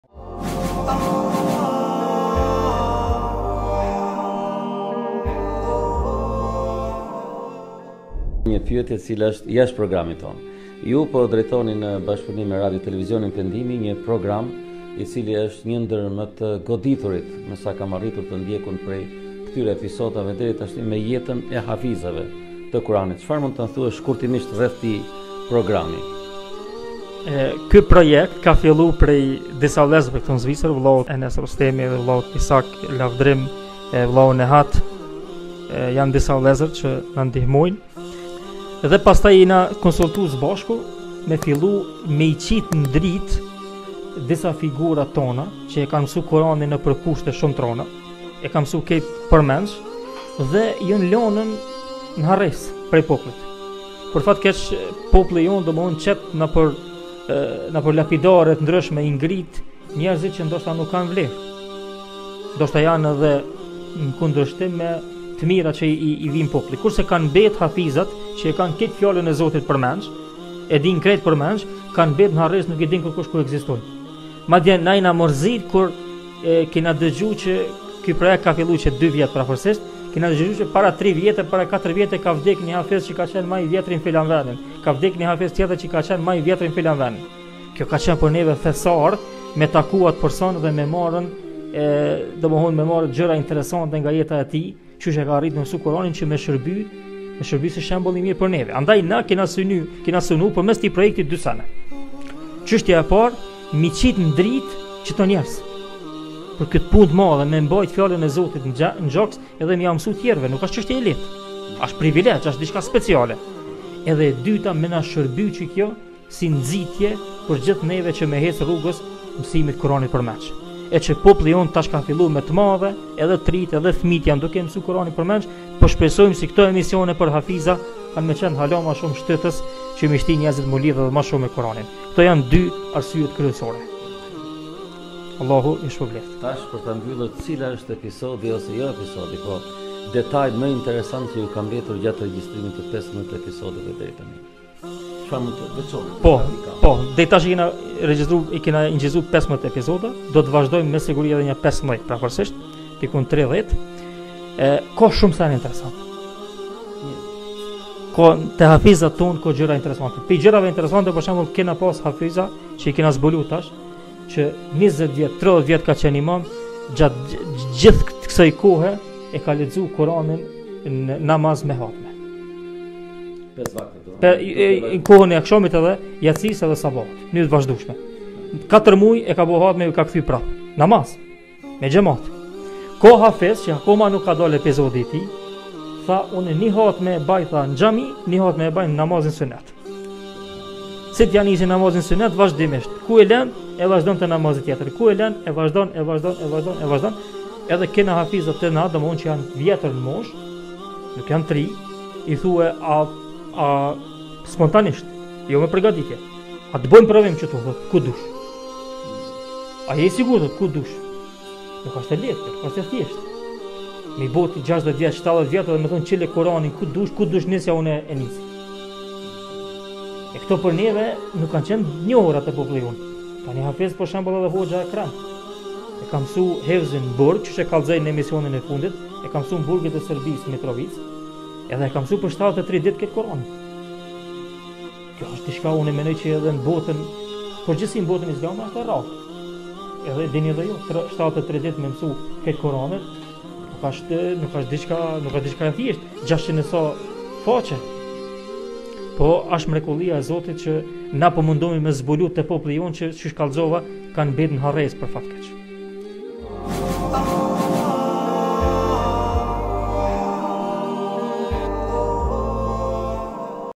Çfarë zbulova nga emisionet... ❤️❤️| Sulejman Rustemi & Xheladin Leka
Një intervist spontane dhe aktuale që u mbajt nga Hoxha Sulejman Rustemi me Hoxhën e nderuar Xheladin Leka.

Shkeputje nga Livestream në FB